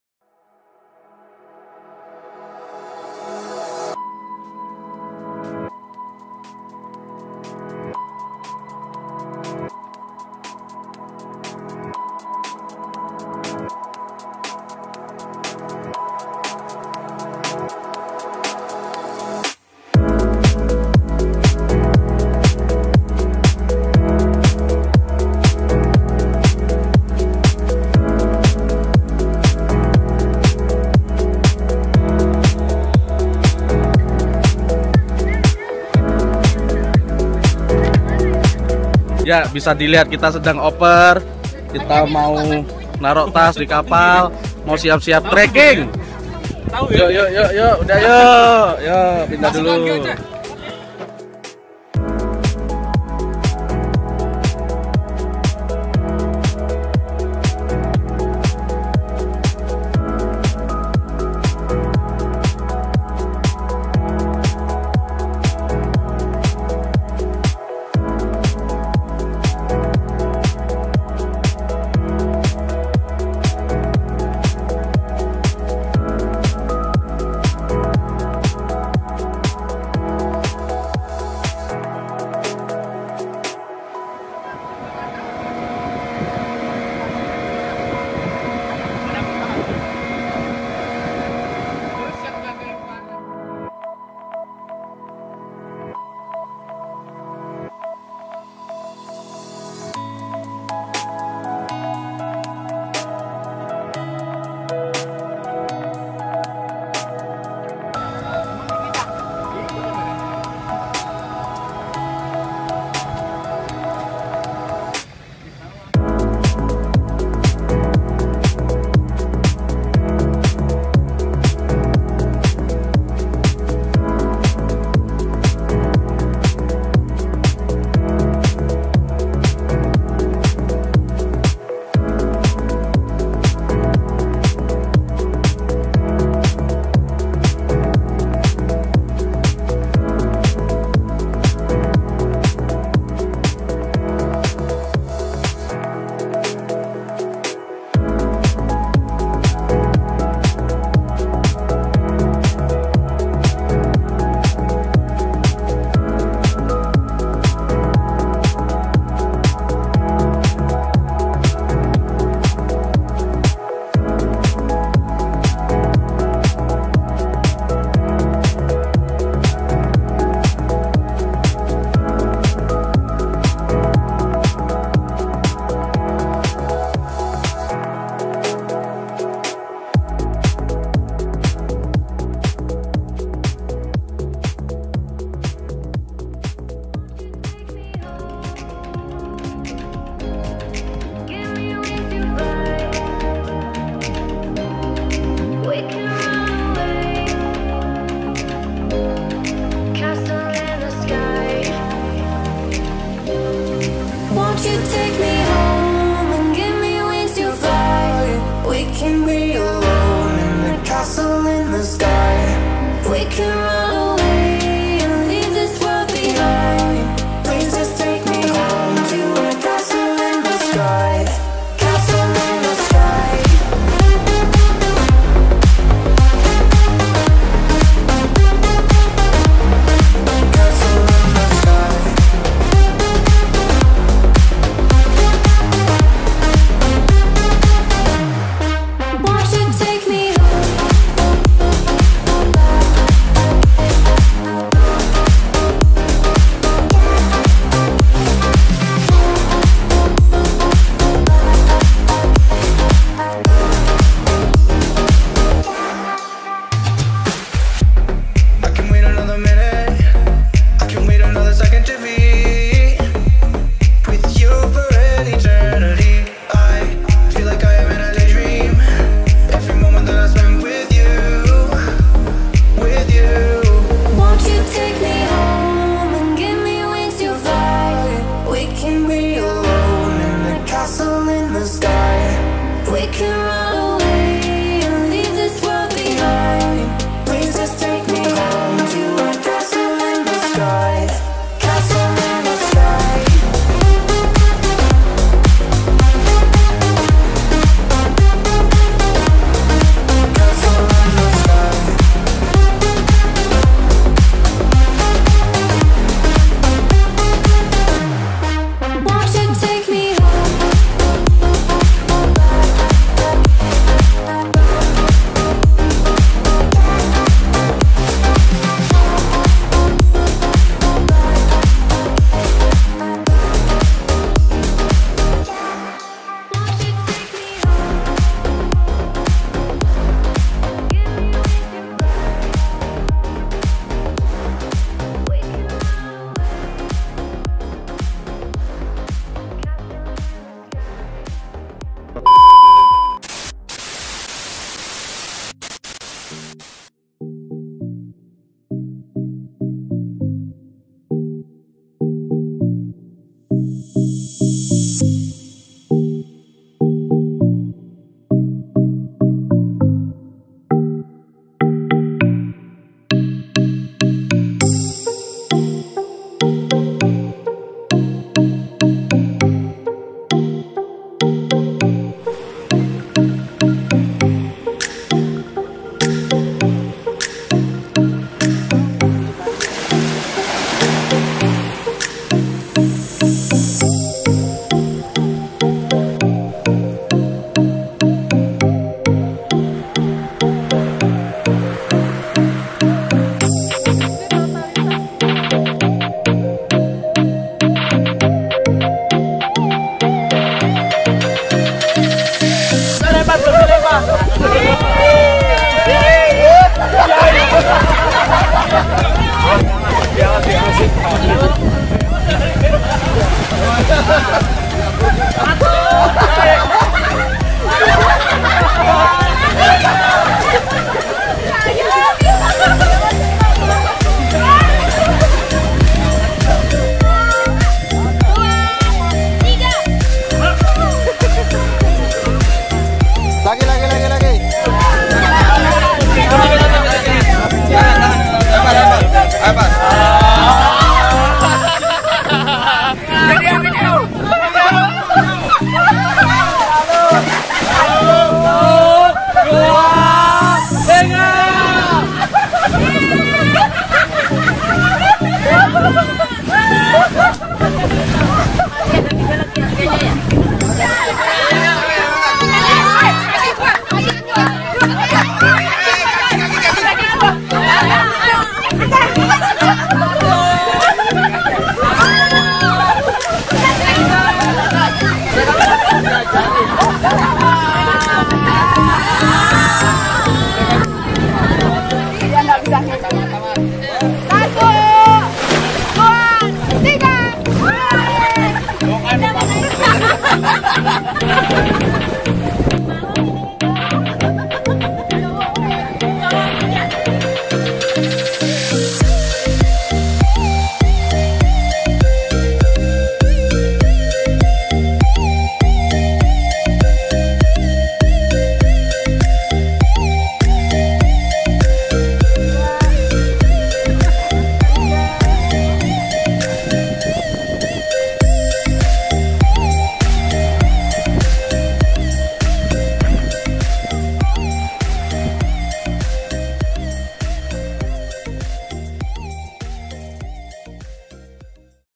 OUTBOND DI GILI GEDE, SEKOTONG, LOMBOK | MENGINAP SEMALAM DI THAMARIND RESORT
Video kali ini menampilkan dokumentasi saat outbound bersama teman teman kantor di Gili Gede, Sekotong Lombok Barat, dan menginap semalam di Resort Thamarind. Resort ini berada di area Gili Gede. Keesokan harinya snorkling dan main air di Gili Layar, Sekotong, tidak jauh dari Gili Gede